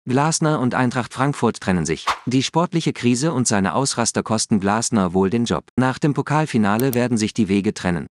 Glasner und Eintracht Frankfurt trennen sich 😱❌
Richtige Entscheidung? 👀